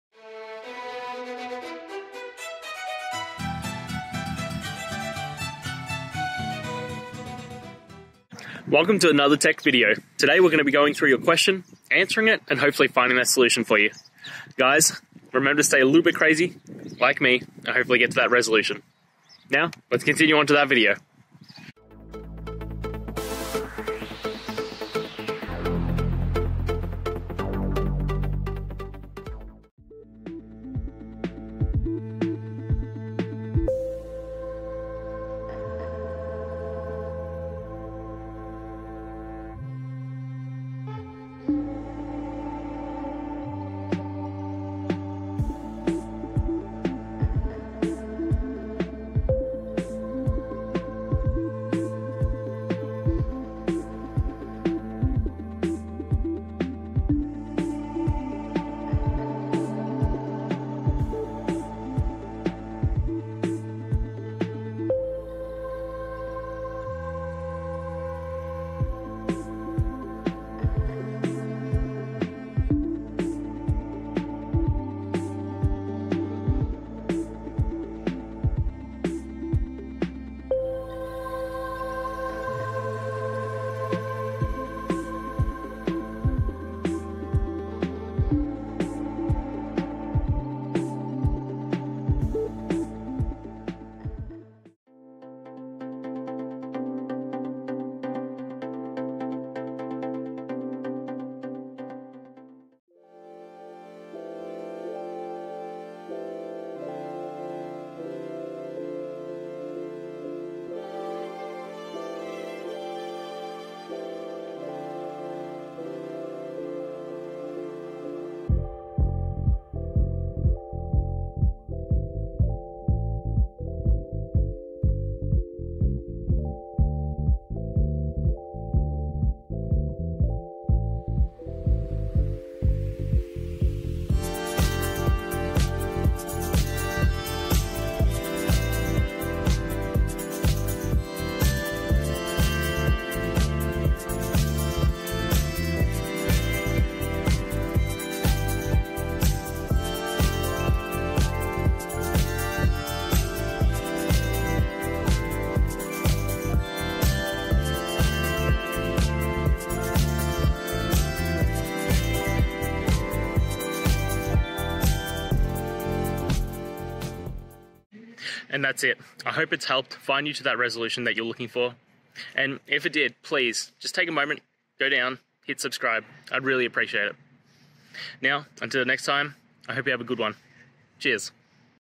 How to Do a Mapping of Array of Strings in Elasticsearch
elasticsearch: How to Do a Mapping of Array of Strings in Elasticsearch

Related to: elasticsearch, clojure, elasticsearch-5, elasticsearch-mapping